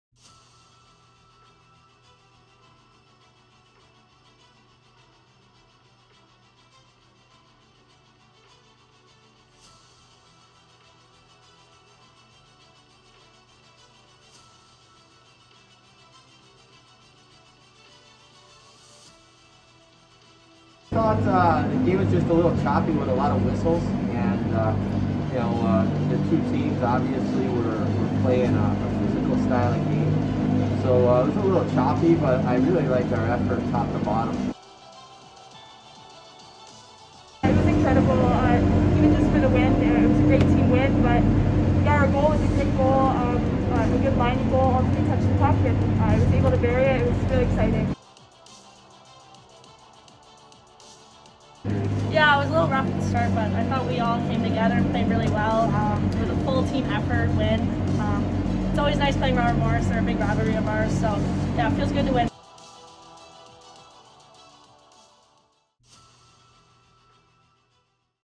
Women's Ice Hockey Downs Robert Morris 4-2
Highlights and interviews with Coach Sisti, Grace Nelles and Summer-Rae Dobson.